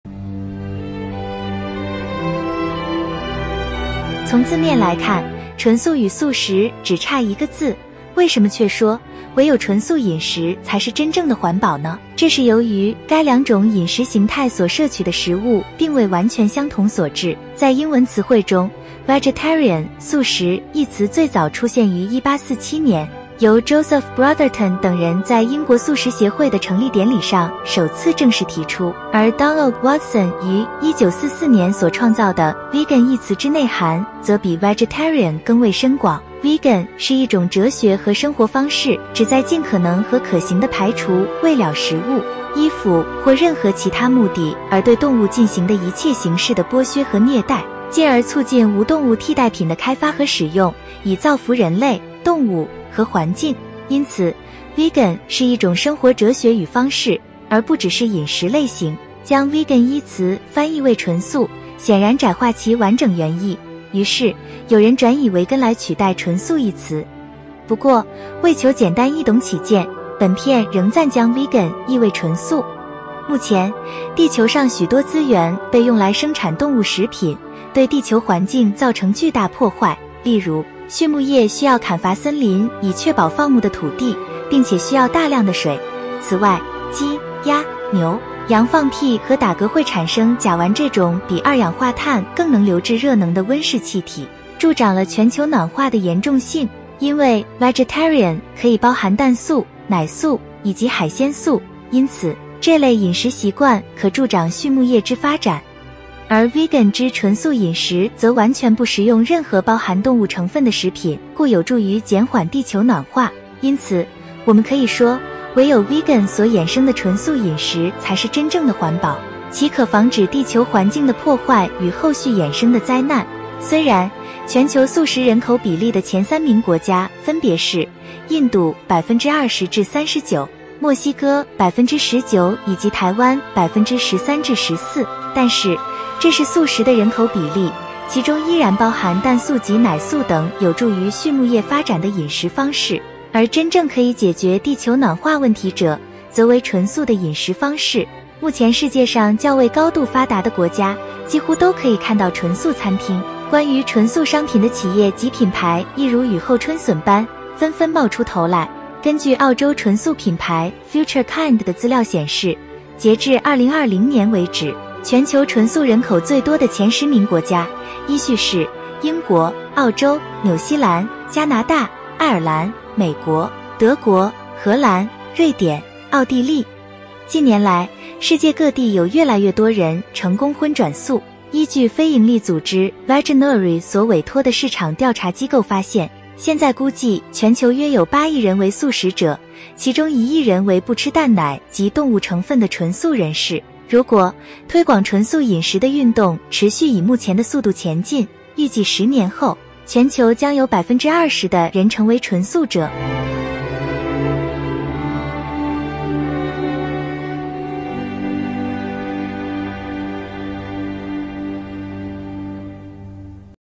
2024.01.24 為何唯有「純素飲食」才是真正的環保？ (Why Only "Being Vegan" Is Truely Environmentally Friendly?)
「Vegetarian」可以包含「蛋素」、「奶素」、以及「海鮮素」，因此，這類飲食習慣可助長「畜牧業」之發展。而「Vegan」之「純素飲食」則完全不食用任何包含動物成分的食品，故有助於減緩「地球暖化」。因此，我們可以說：唯有「Vegan」所衍生的「純素飲食」才是真正的環保，其可防止「地球環境的破壞」與「後續衍生的災難」。

"Vegetarian" can include "egg-vegetarian", "lacto-vegetarian", and "seafood-vegetarian". Therefore, this type of eating habits can promote the development of "animal husbandry". The "vegan diet" does not consume any food containing animal ingredients, so it helps slow down "global warming". Therefore, we can say that only the "vegan diet" derived from "Veganism" is truly environmentally friendly, which can prevent "destruction of the global environment" and "subsequent disasters."

Be Veg, Go Green, to Save our Earth (純素、環保、救地球)